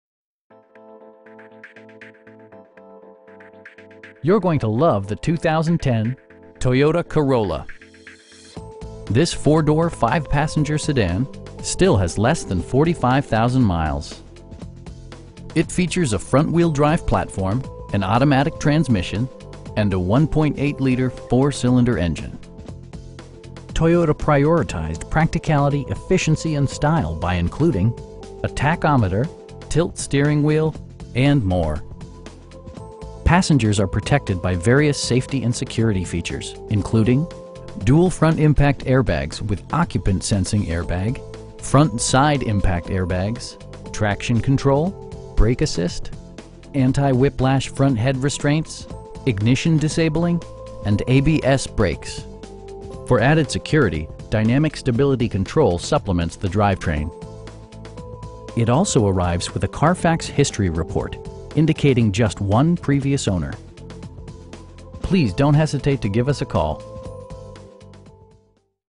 2010 Toyota Corolla in Council Bluffs, IA 51501-8017
You're going to love the 2010 Toyota Corolla. This 4 door, 5 passenger sedan still has less than 45,000 miles! It features a front-wheel-drive platform, an automatic transmission, and a 1.8 liter 4 cylinder engine. Toyota prioritized practicality, efficiency, and style by including: a tachometer, front bucket seats, air conditioning, tilt steering wheel, power door mirrors, and more. Passengers are protected by various safety and security features, including: dual front impact airbags with occupant sensing airbag, front SIDE impact airbags, traction control, brake assist, anti-whiplash front head restraints, ignition disabling, and ABS brakes. For added security, dynamic Stability Control supplements the drivetrain. It also arrives with a Carfax history report, indicating just one previous owner. We offer competitive pricing, and the area's best shopping experience. Please don't hesitate to give us a call.